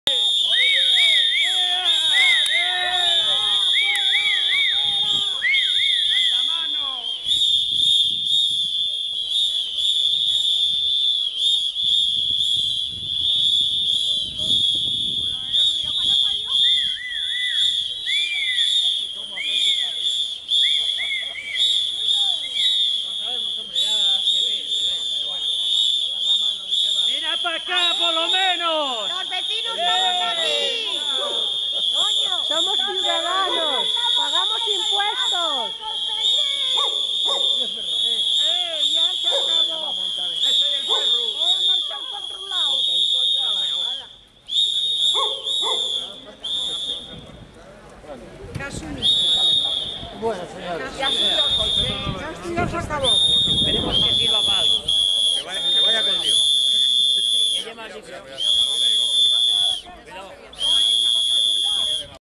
PITADA A ARECES_1
MAYO 2011: La PLATAFORMA PARA EL ENLACE CON LA AS-2 se manifiesta con una sonora pitada en la inauguración del INSTITUTO DE MEDICINA LEGAL en La Corredoria al que acudió el Sr. Areces y otras autoridades